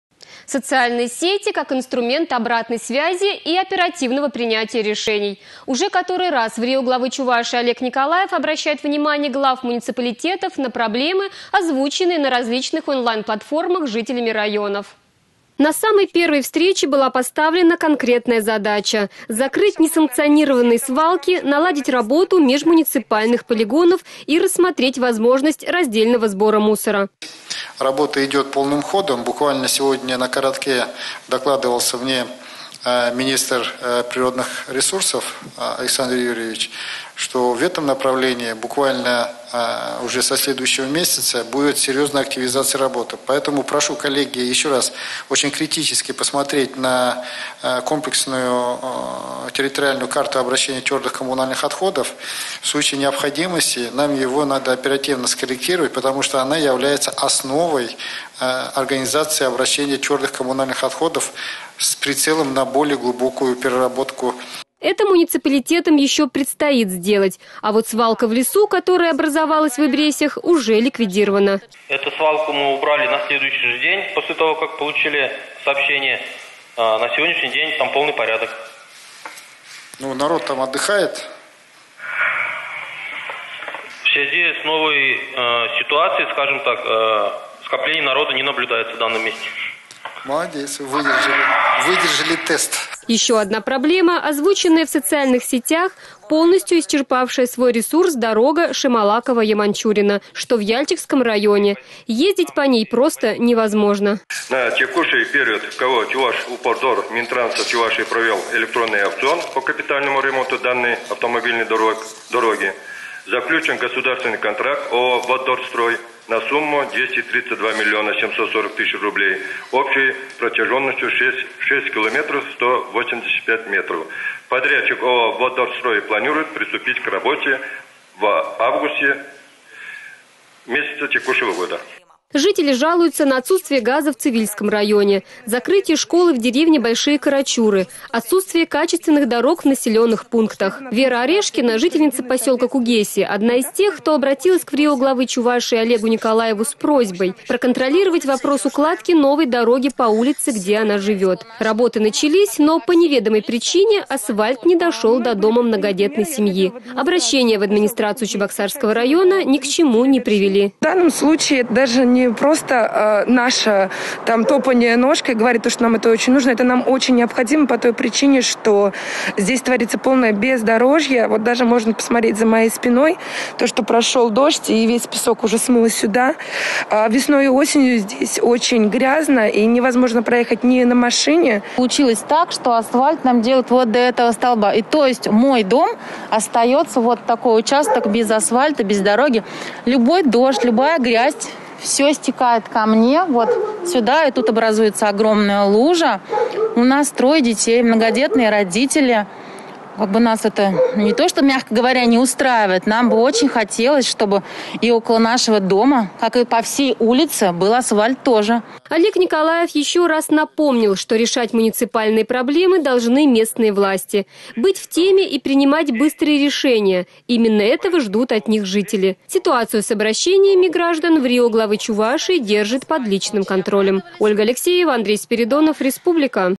Врио Главы Чувашии Олег Николаев провел совещание с главами муниципалитетов по обращениям в соцсетях
Уже который  раз  врио Главы Чувашии Олег Николаев обращает внимание глав муниципалитетов на проблемы,  озвученные на различных онлайн платформах жителями  районов.